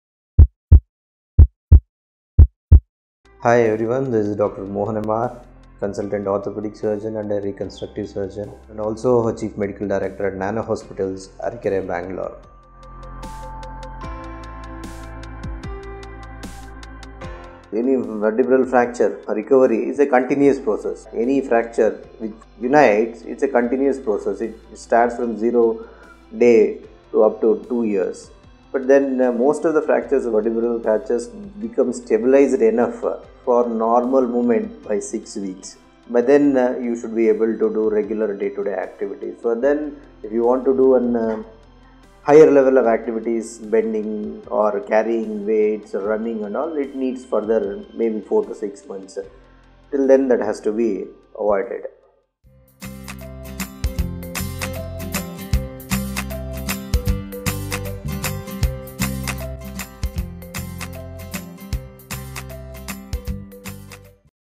Duration to recover from vertebral compression fracture - Dr. Mohan M R
Any vertebral fracture, recovery is a continuous process. Any fracture which unites it is a continuous process. It starts from a day to upto 2 years. But the most of the vertebral fracture becomes stabilized enough or normal movement by 6 week, by then you should be able to do normal day to day activities. If you want to do higher level of activities, bending or carrying weights or running it needs 4 to 6 months, till then that has to be avoided.